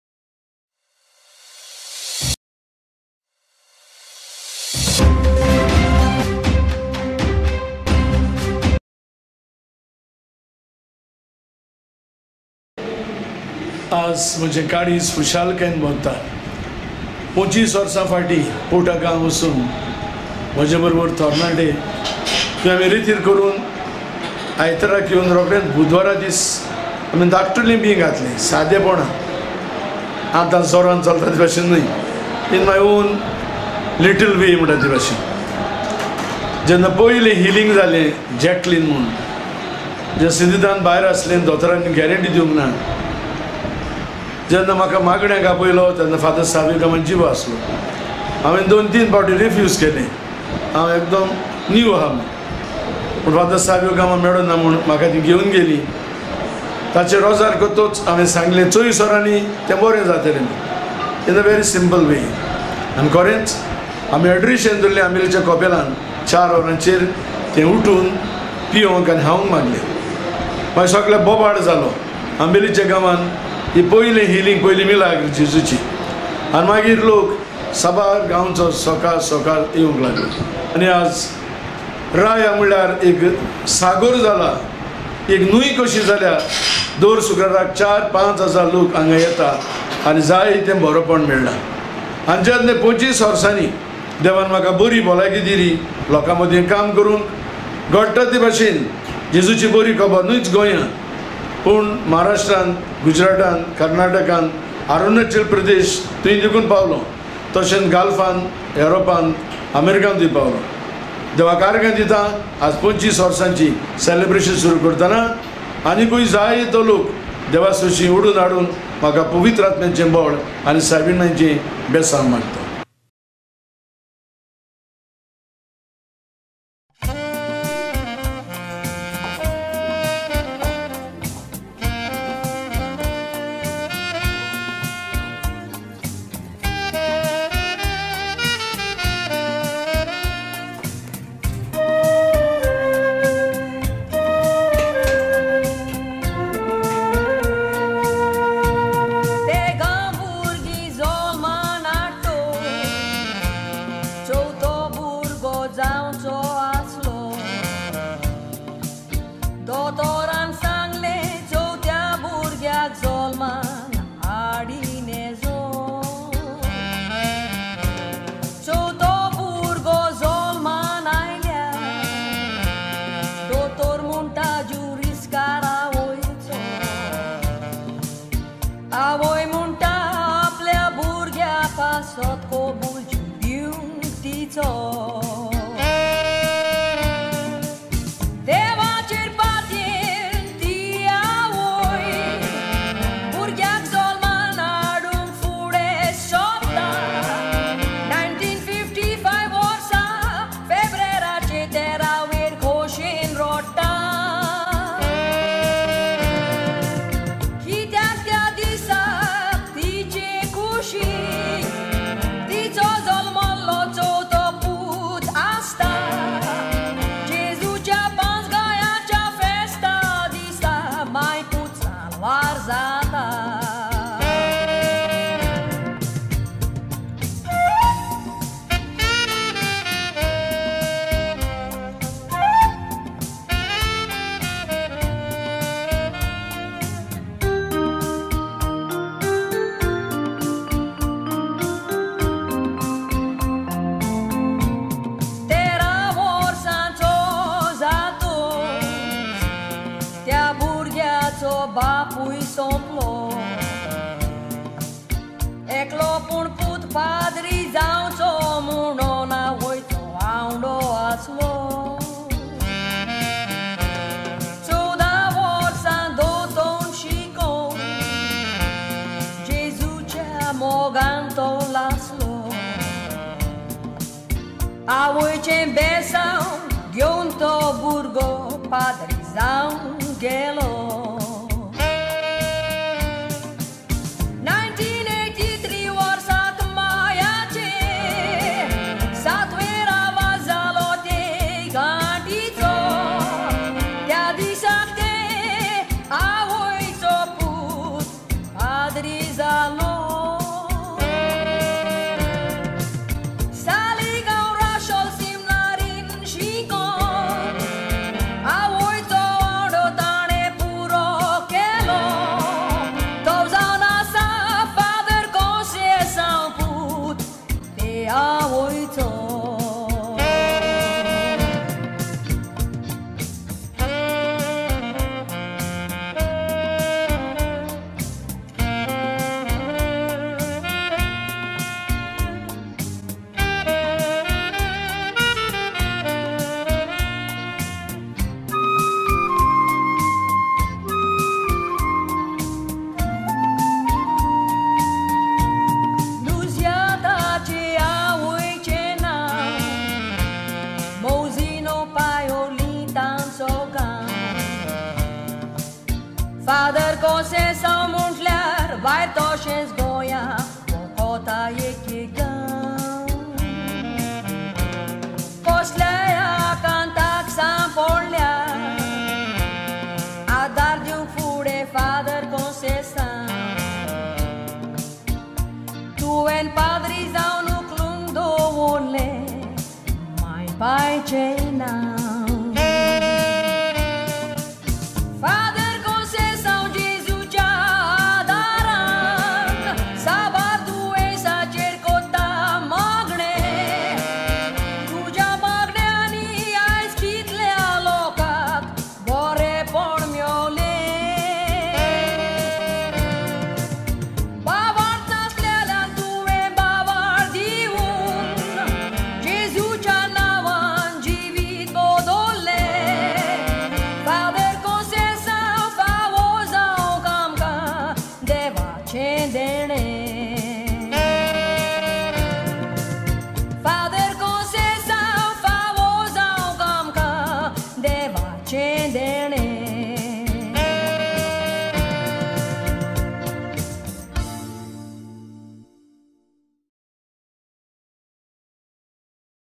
Tribute to Fr Conceicao - Inspirational Words of Fr Conceicao and song by Lorna.Truegoanews
TrueGoaNews Tribute to Fr Conceicao x264